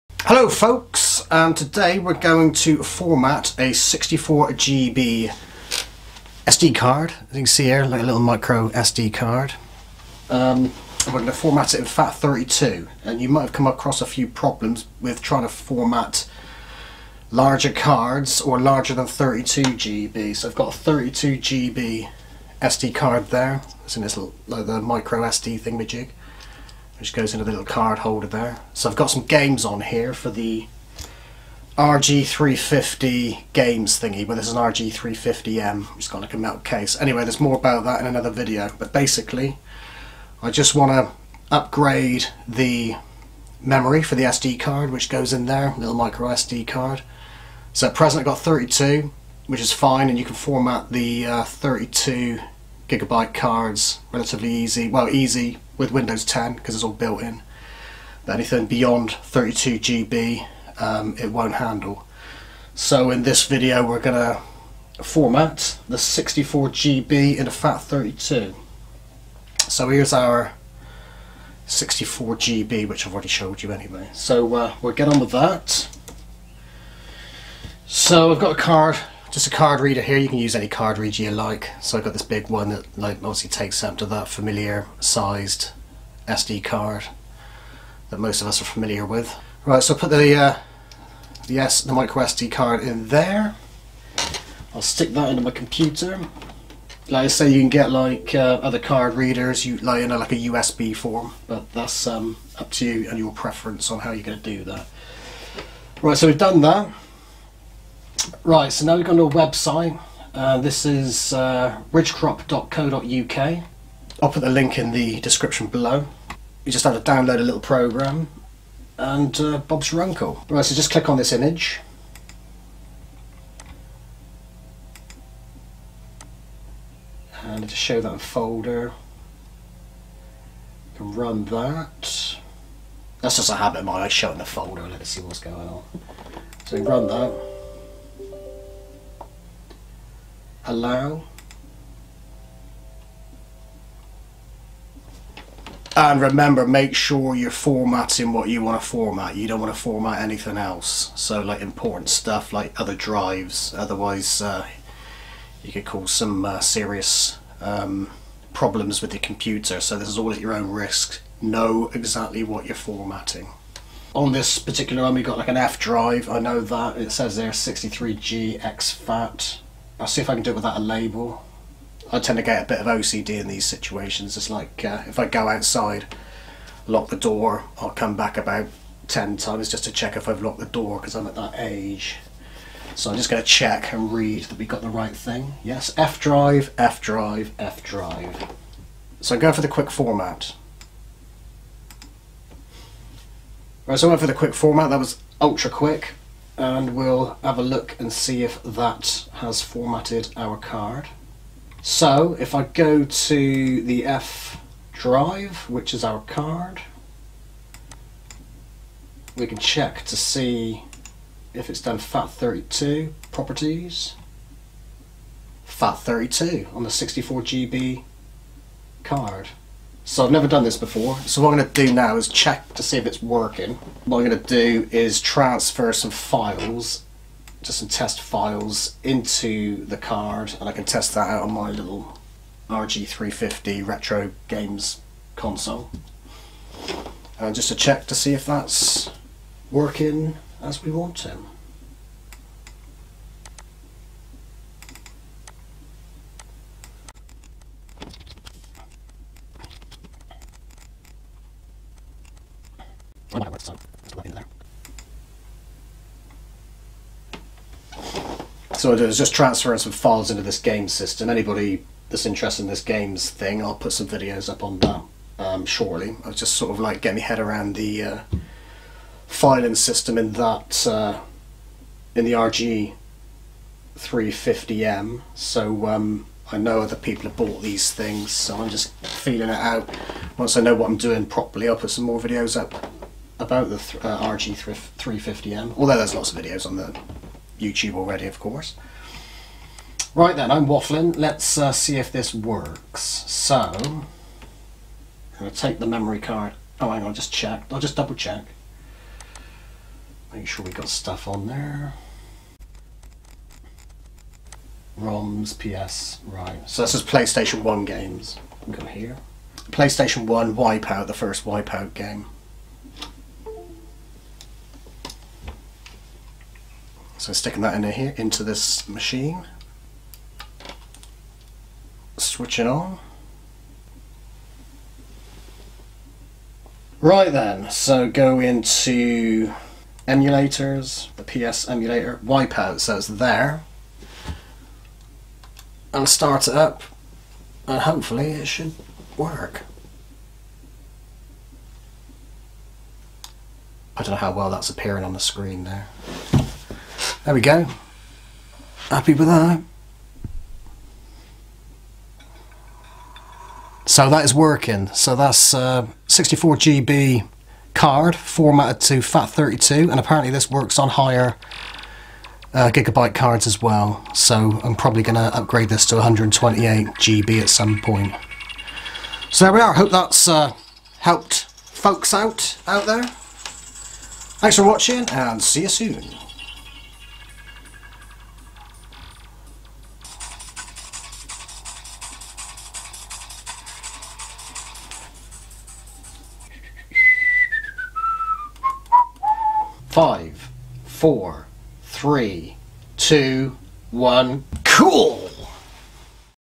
Format 64GB SD Card To FAT32 (Works On Higher GB Cards/Drives Too)
I have recently stumbled onto the problem of wanting higher stotage capacity on an SD card and finding that Windows 10 will only format up to 32GB for FAT32. Anything above requires further methods and one is shown in this video.

Amazon Storefront: